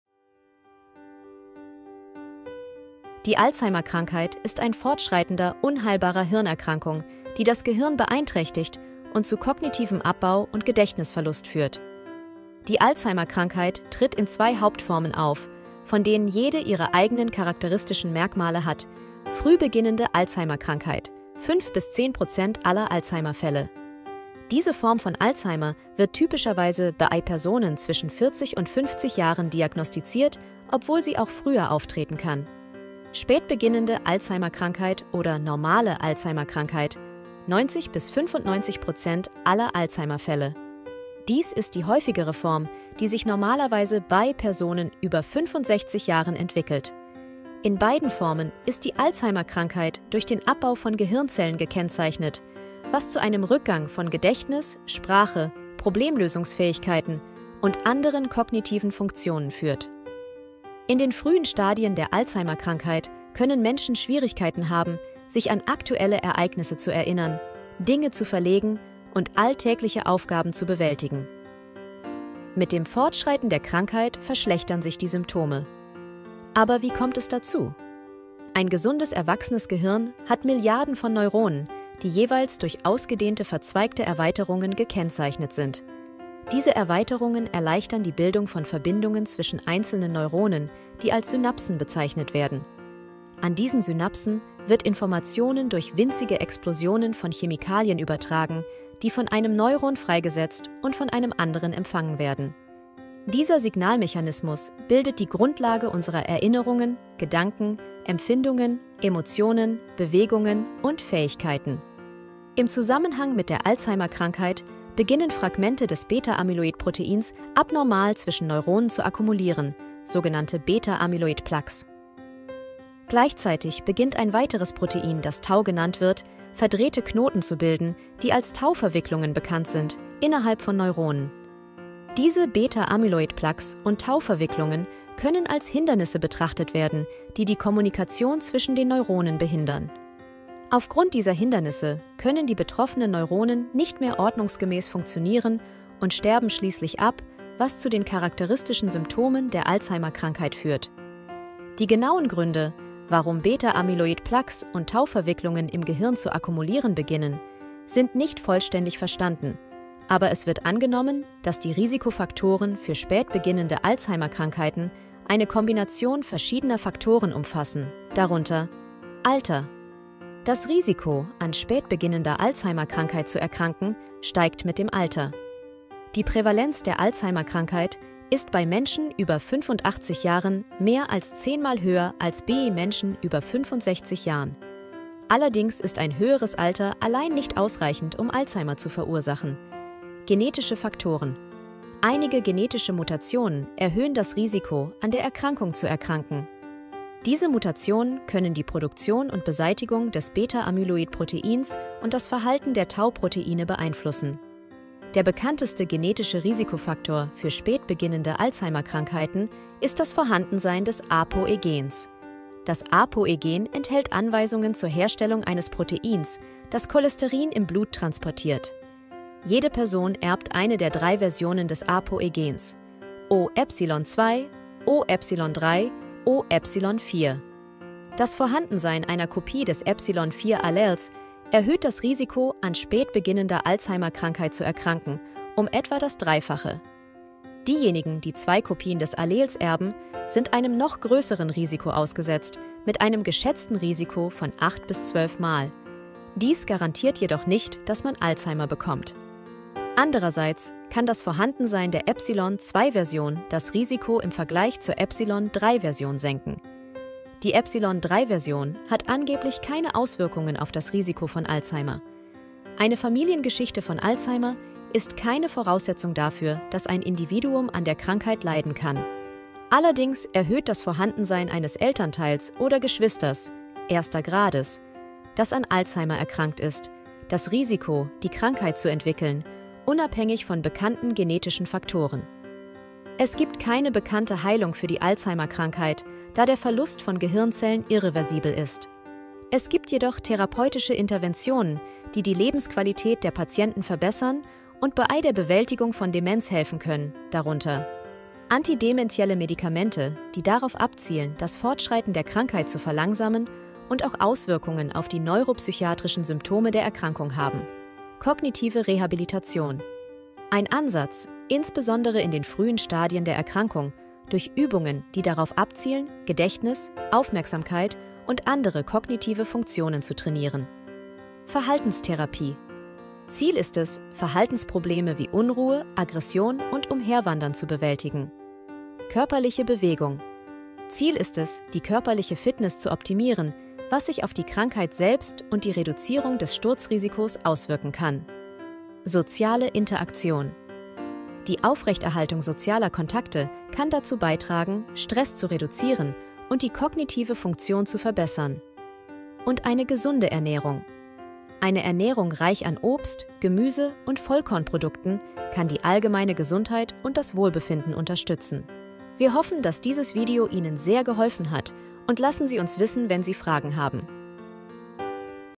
Alzheimer-Krankheit - Meamedica-DNA-Bericht
Kurze Erklärung zur Alzheimer-Krankheit und ihren genetischen Komponenten.